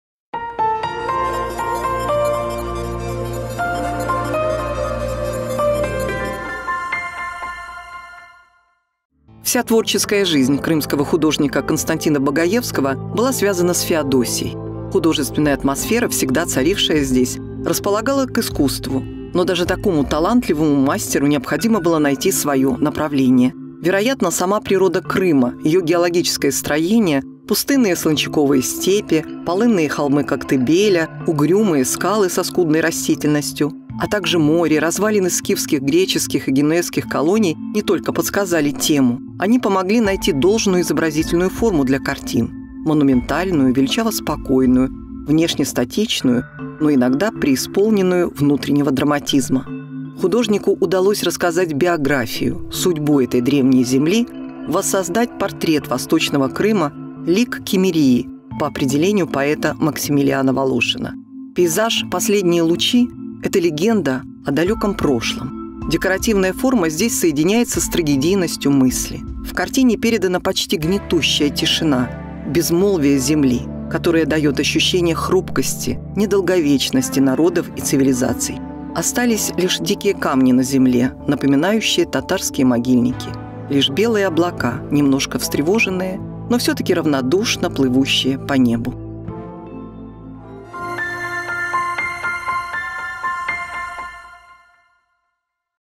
История одной картины. «Последние лучи»
В этом выпуске — обзор картины «Последние лучи». Художник Константин Богаевский.